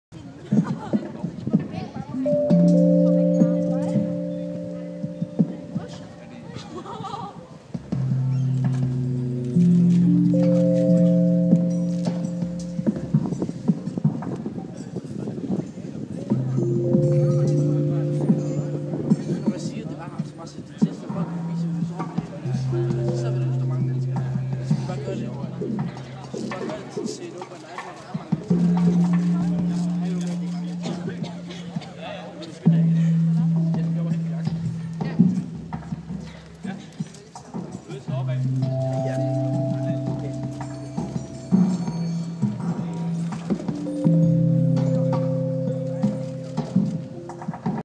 3D-lysshow på Søndergade
Mærk Byen har haft premiereweekend på sit 3D-lysshow, hvor Bytorvets facade bliver brugt til lærred for stor computergrafik - en grafik som tilskuerne via bevægelser kan blive en del af.